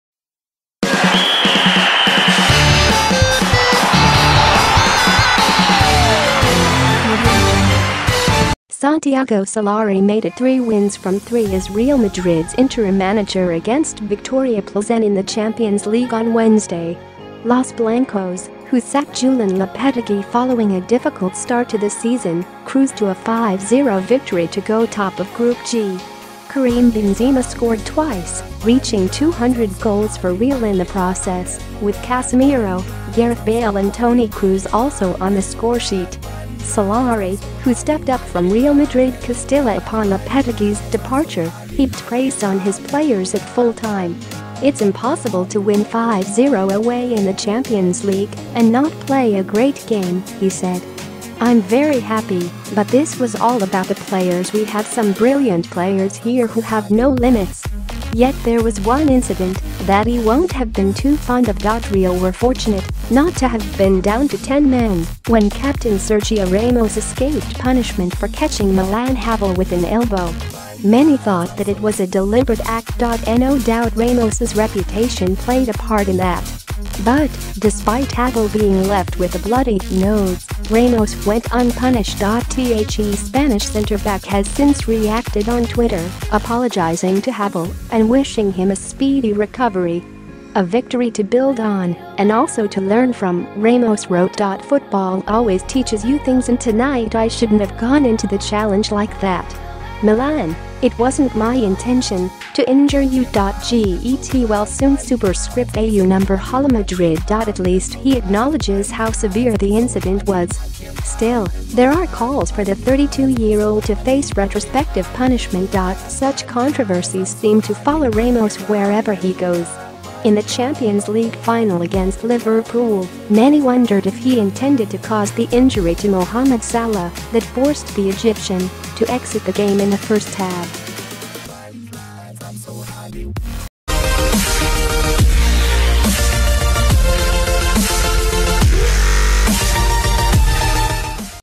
Sergio Ramos has tweeted after being accused of elbowing Viktoria Plzen's Milan Havel
Santiago Solari made it three wins from three as Real Madrid’s interim manager against Viktoria Plzen in the Champions League on Wednesday.Los Blancos, who sacked Julen Lopetegui following a difficult start to the season, cruised to a 5-0 victory to go top of Group G.Karim Benzema scored twice, reaching 200 goals for Real in the process, with Casemiro, Gareth Bale and Toni Kroos also on the scoresheet.Solari, who stepped up from Real Madrid Castilla upon Lopetegui’s departure, heaped praise on h :.